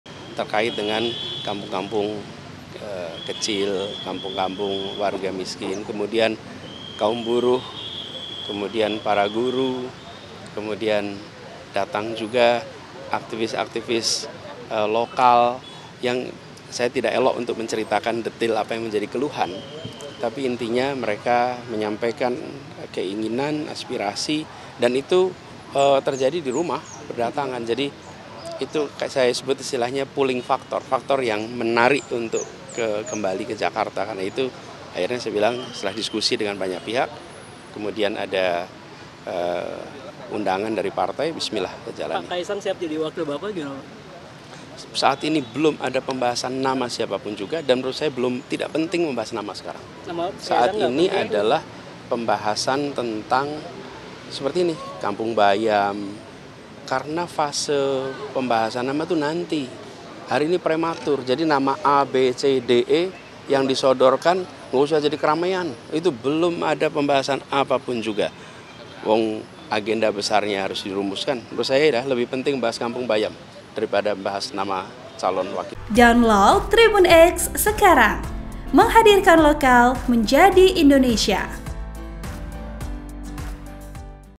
MAJU Pilkada, Anies Ogah Bahas Nama Cawagub Jakarta: Lebih Penting Bahas Kampung Bayang
TRIBUN-MEDAN.COM - Mantan Gubernur DKI Jakarta, Anies Baswedan mengatakan bahwa lebih penting untuk membahas Kampung Bayam daripada Bahas nama Calon Wakil Gubernur untuk Pilkada DKI Jakarta.

Editor Video : Jihan Fikriyah